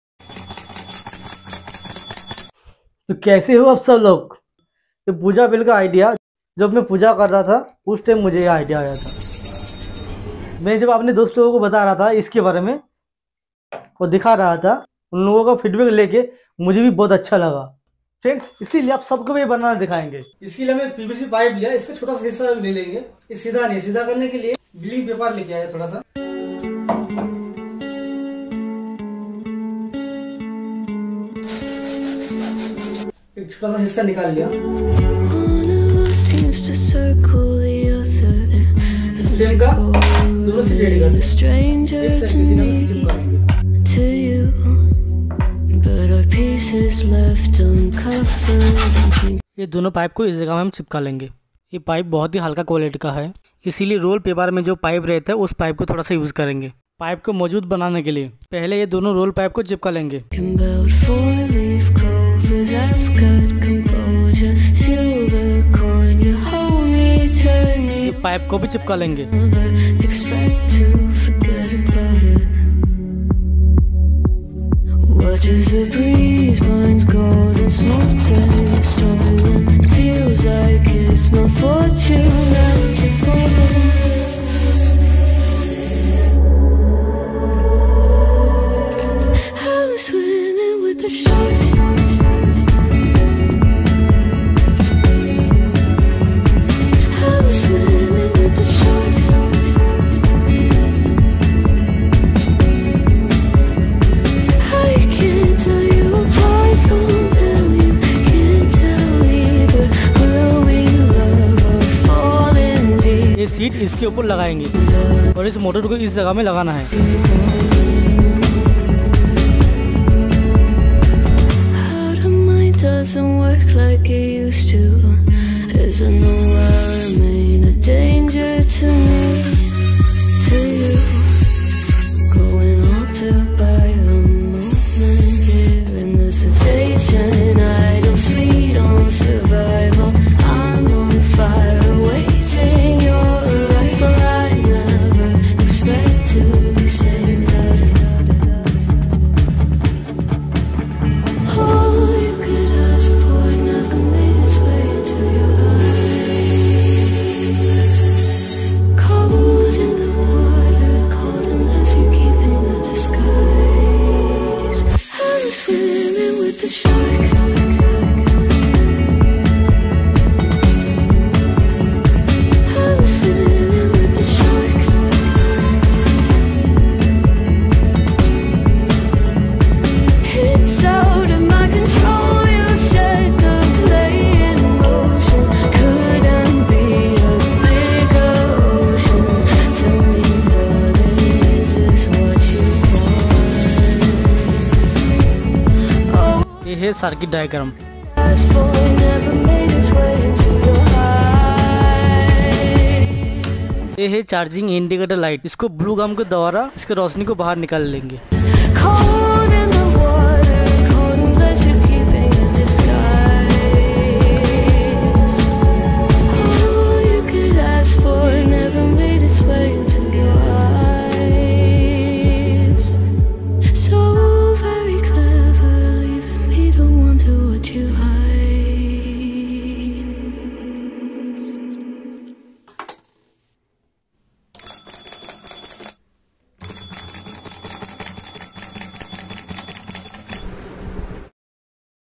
How to Make Automatic Pooja Bell At Home
material: ---
           5 V DC motor
           battery 🔋
           Electric pipe
           pvc pipe
           Pooja Bell🔔
          3.7 v battery 🔋 BMS
          wire
my angel Grinder-.

universal rotary tools-

soldering iron

Automatic Pooja ghanti video description

An automatic Pooja ghanti is a device that produces the sound of a bell or gong traditionally used in Hindu worship ceremonies, without requiring physical manual effort to ring the bell. The device typically consists of an electronic circuit that controls a motor or solenoid to strike the bell or gong.

In this video, we can see a demonstration of an automatic Pooja ghanti in action. The video may start by showing the device itself, highlighting its key features such as its size, shape, and construction materials. The device is then switched on, and we can hear the sound of the bell being produced automatically.

The video may also show how the device is installed or mounted in a Pooja room or temple, and how it can be controlled either by a switch or remote control. The benefits of using an automatic Pooja ghanti, such as convenience and consistency of sound, may also be highlighted.

Overall, the video aims to showcase the features and benefits of an automatic Pooja ghanti, and how it can be a useful addition to any Hindu worship space.

Background music